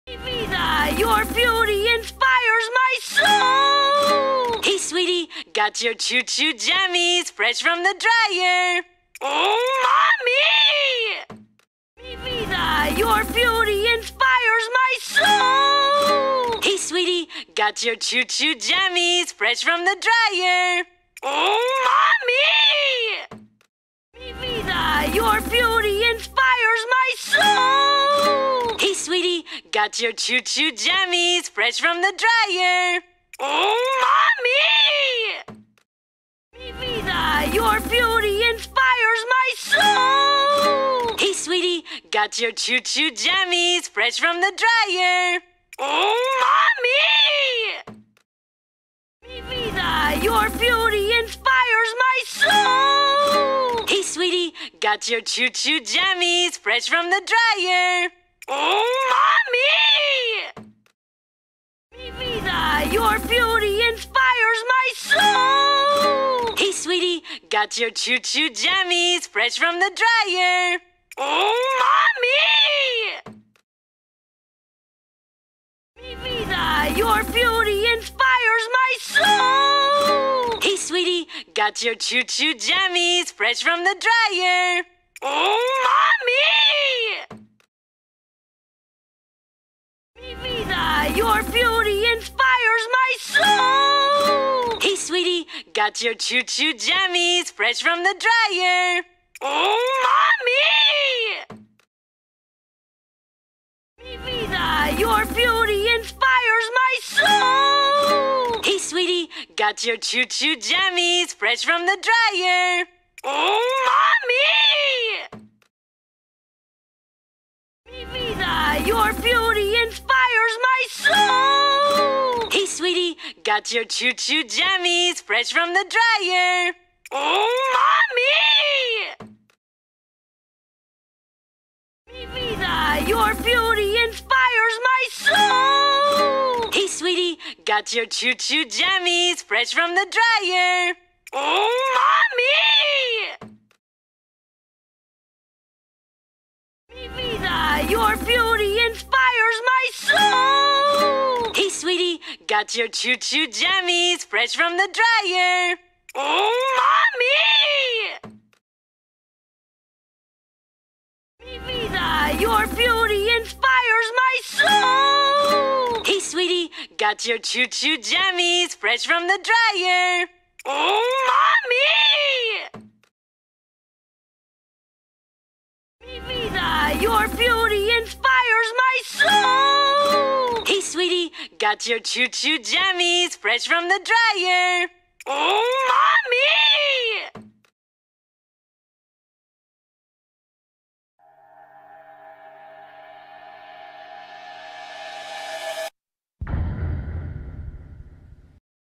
Carl shows up more times than there are grains of sand on the entire Earth!
Timelines:
0:00 - 1
0:12 - 49
0:24 - 2,401
0:36 - 117,649
0:49 - 5,764,801
1:02 - 282,475,249
1:16 - 13,841,287,201
1:30 - 678,223,072,849
1:44 - 33,232,930,569,601
1:58 - 1,628,413,597,910,449
2:12 - 79,792,266,297,612,001
2:27 - 3,909,821,048,582,988,049
2:41 - 191,581,231,380,566,414,401
2:56 - 9,583,052,844,682,082,520,349
3:11 - Total